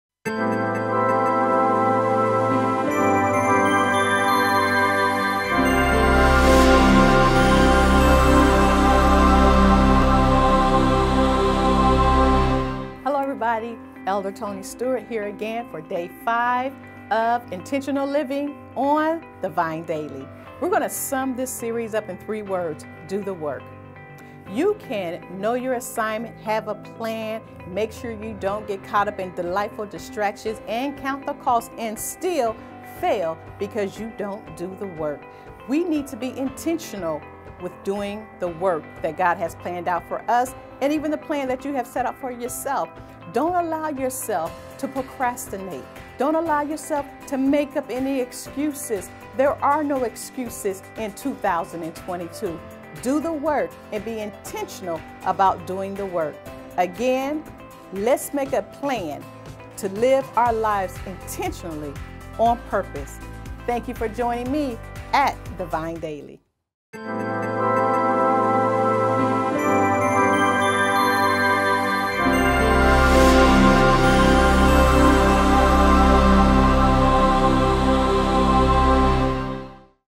The Vine Daily
Welcome to The Vine Daily! Enjoy the last installment of Elder Toni's devotional on "Intentional Living".